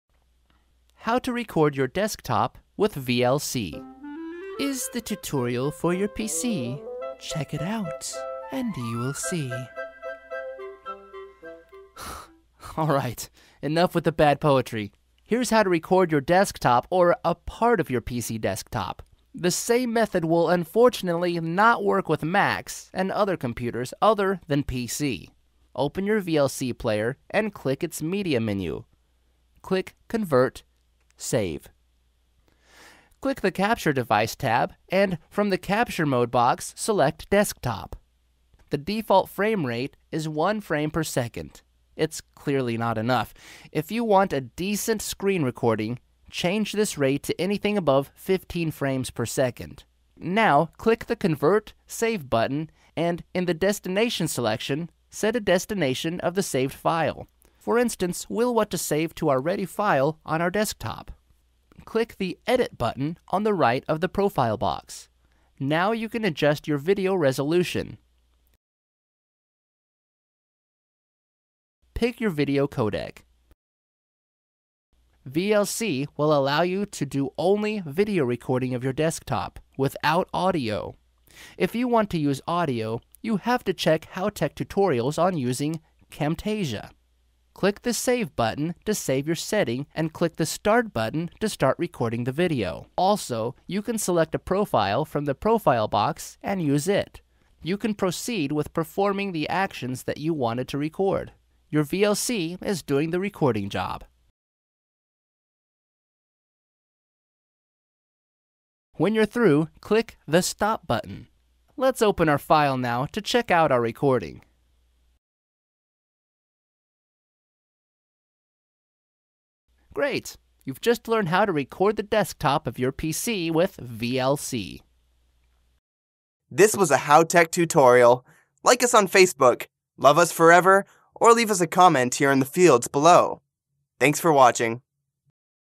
How to Record Your Desktop with VLC
How to record your desktop with VLC is the tutorial for your PC. Check it out and you will see!

All right...enough of bad poetry:) Here's how to record your desktop or a part of your PC desktop. The same method will unfortunately not work with Macs and computers other than PC.

Step 1. Open your VLC player and click its "Media" menu. Select "Convert/Save".

Step 2. Click the "Capture Device" tab and, from the "Capture" mode box, select "Desktop".

Step 3. The default frame rate is one frame per second. It's clearly not enough. If you want a decent screen recording, change this rate to anything above 15 frames per second.

Step 4. Now click the "Convert/Save" button and, in "Destination" section, set a destination of the saved file. For instance, we'll want to save our ready file on our desktop.

Step 5.  Click the "Edit" button on the right of the "Profile" box. Now you can adjust your video resolution. Pick your video codec.

Step 6. VLC will allow you to do only video recording of your desktop, without audio. If you want to use audio, you have to check other HowTech tutorials on using Camtasia.

Step 7. Click the "Save" button to save your setting and click the "Start" button to start recording the video. Also you can select the profile from the "Profile" box and use it.

Step 8. You can proceed with performing the actions that you wanted to record - your VLC is doing the recording job.

Step 9. When you're through, click the  "Stop" button.

Result: Let's open now the saved file to check our recording. Great! You've just learned how to record the desktop of your PC with VLC.